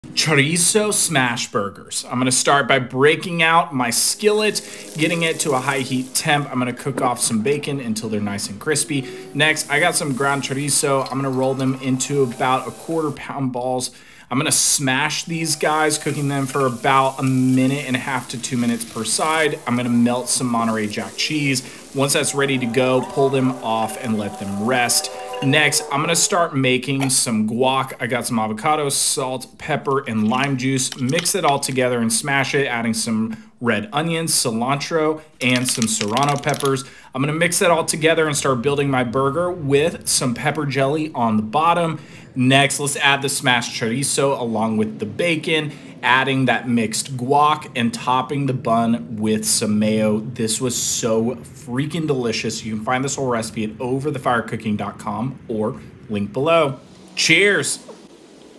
Chorizo Smashburger 🍔🌶🔥
Chorizo Smashburger will change your world 🌎🍔🌶🔥

Chorizo Smashburgers are everything we love about chorizo from @nationalporkboard and smashburgers, now combined for the ultimate bun burger experience! Topped with fresh guacamole and bacon, this chorizo smashburger is a cool, spicy experience that will keep you coming back for more. Oh, and did I mention pepper jelly? Cuz that’s in there too. Yeah, I know you want to hear more about this one. Read on, my hungry friends.